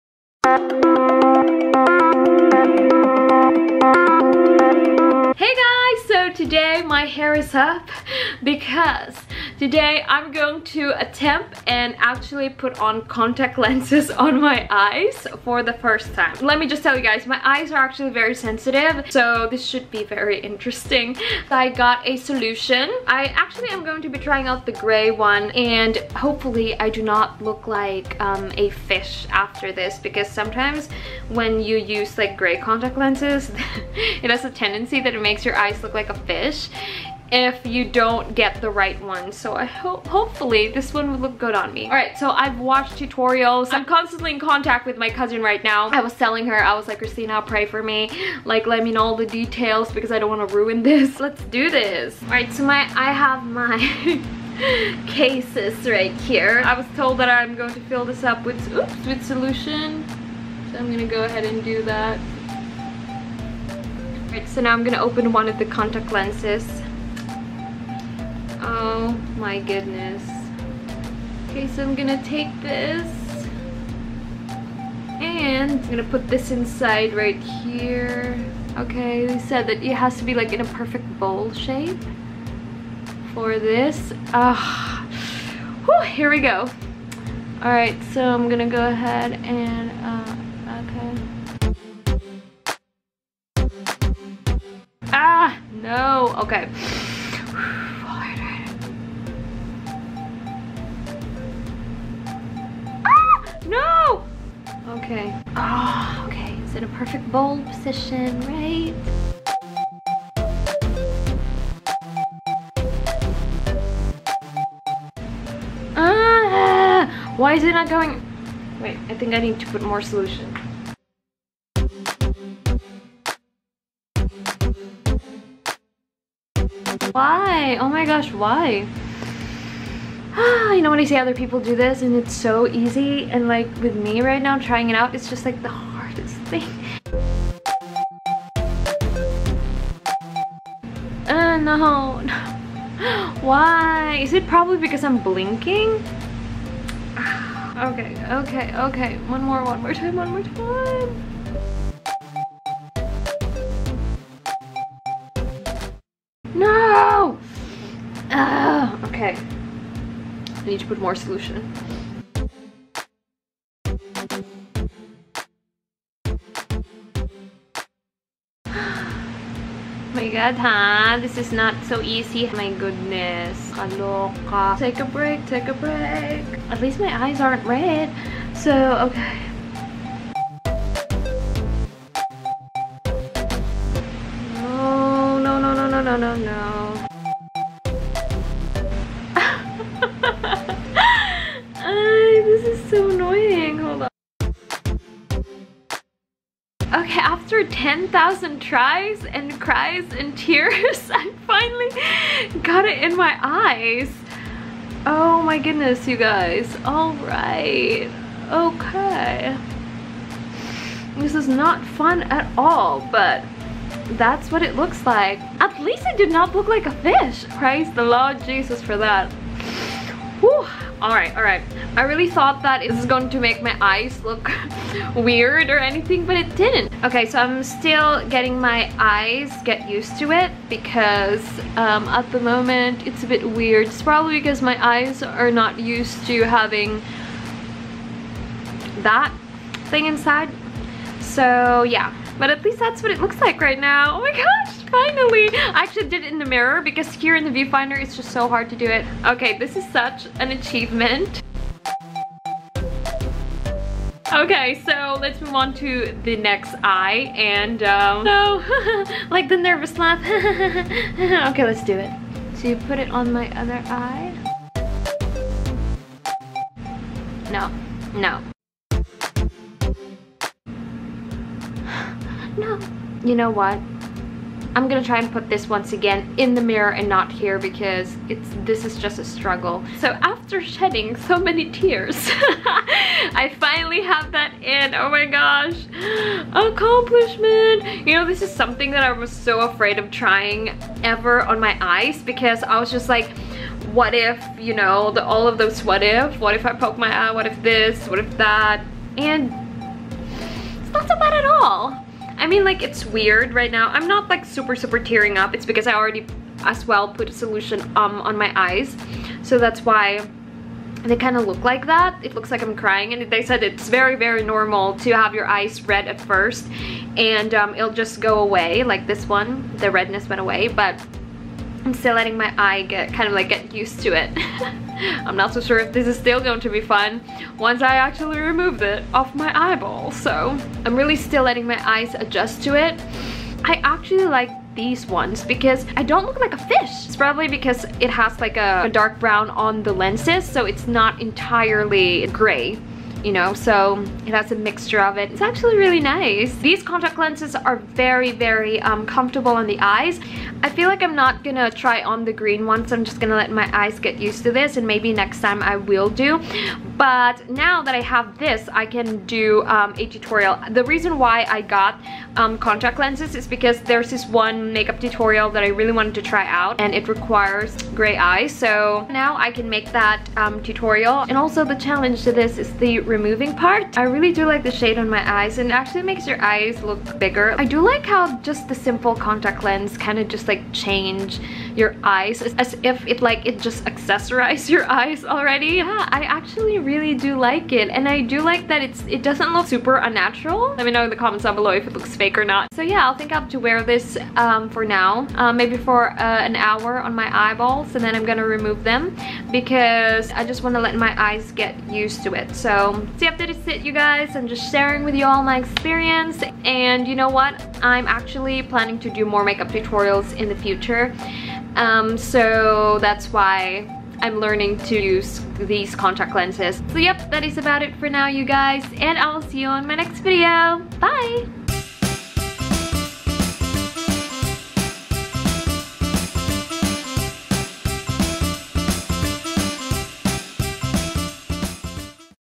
My First Time Putting On Contact Lenses- The Struggle & The Pain-Brown Eyes To Grey - Beautyklove
My Favorite Hair Growth Oils-

My  Hair Mask Recommends-

My Organic Shampoo & Conditioner Recommends-